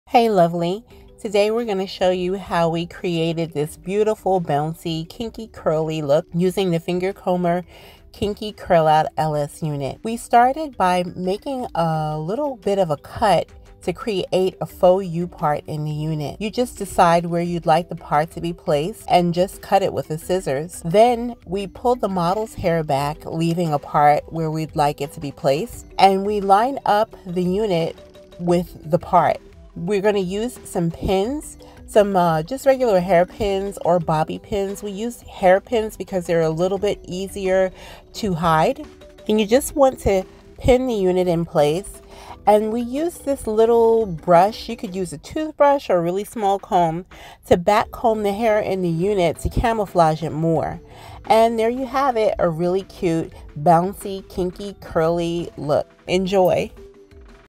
FINGERCOMBER KINKY KURL OUT LS Unit!!!
If you dont have time to Cornrow your hair, AND dry out your hairline with alcohol, AND glue stockings to your cornrows, AND put concealer on the stockings, AND glue a lace front to the stocking? You'll love the FINGERCOMBER KINKY KURL OUT LS Unit!!! You can rock it with a part with. NOLEAVEOUT! 💙Takes less than a minute to install💛 💛